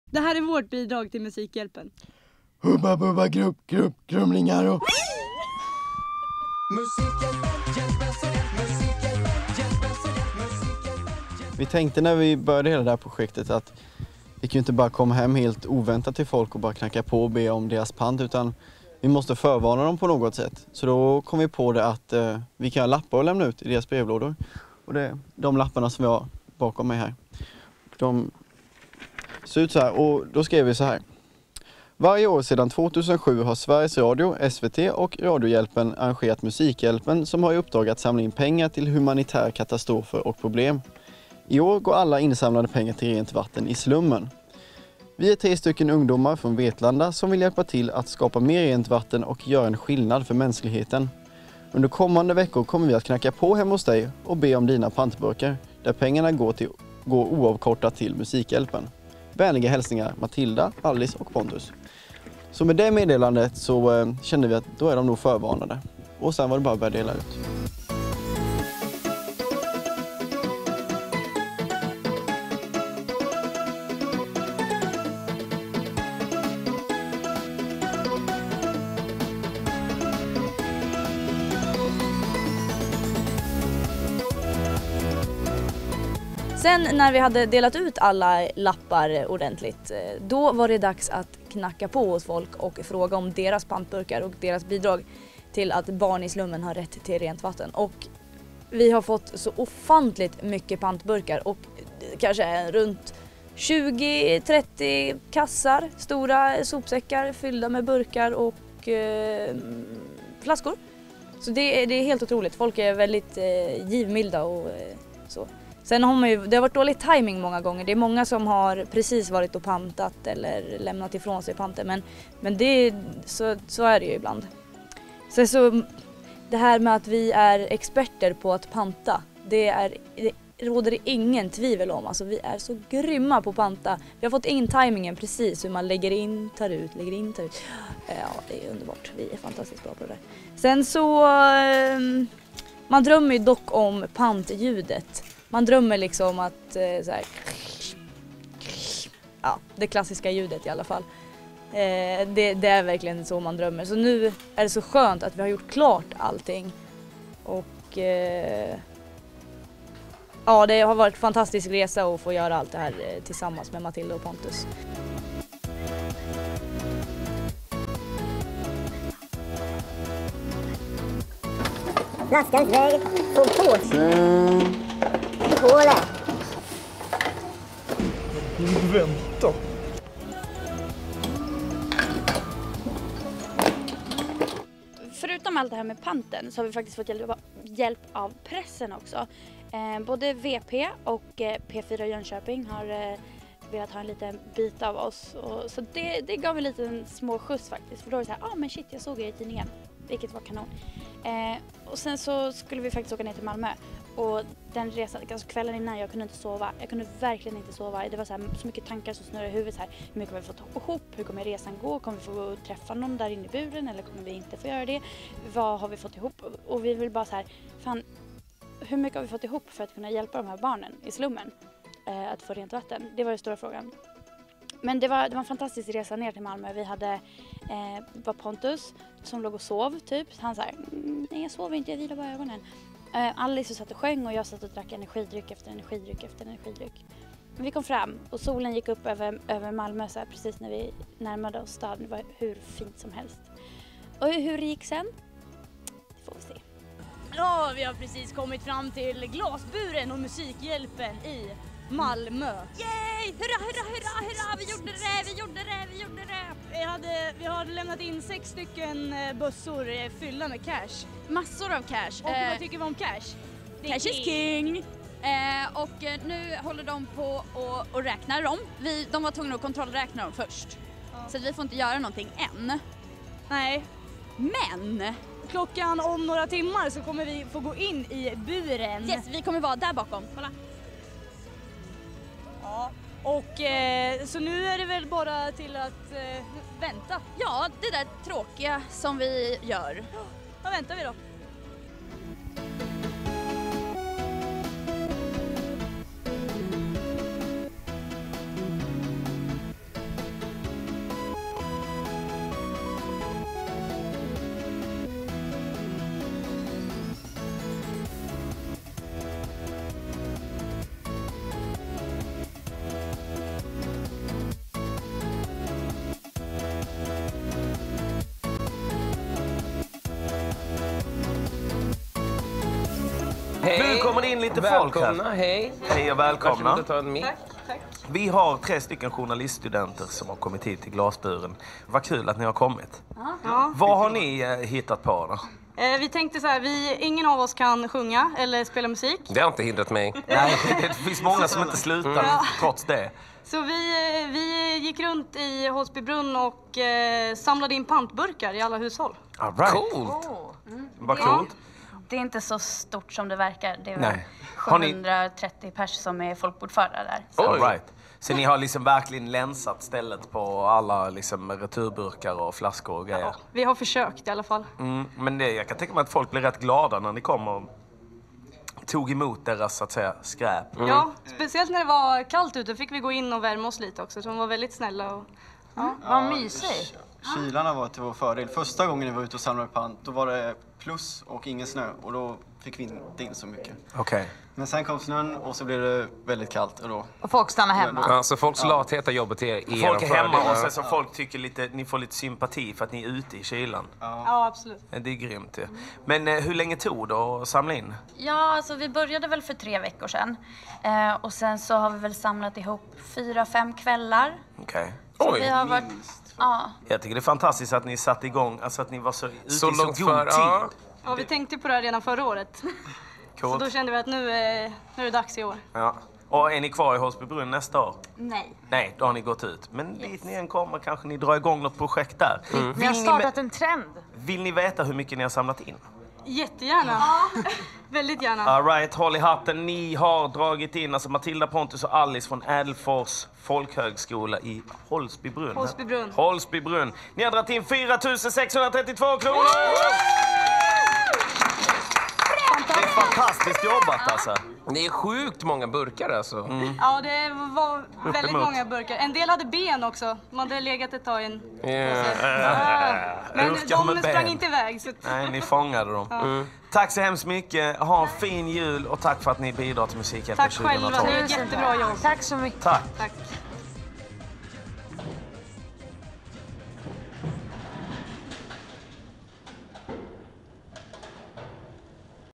Musikhjälpen-HJÄLPEN
Tre elever på Ädelfors folkhögskola går journalistutbildningen och ville bidra till musikhjälpen 2012. De samlade ihop pant i alla hushåll från Holsbybrunn!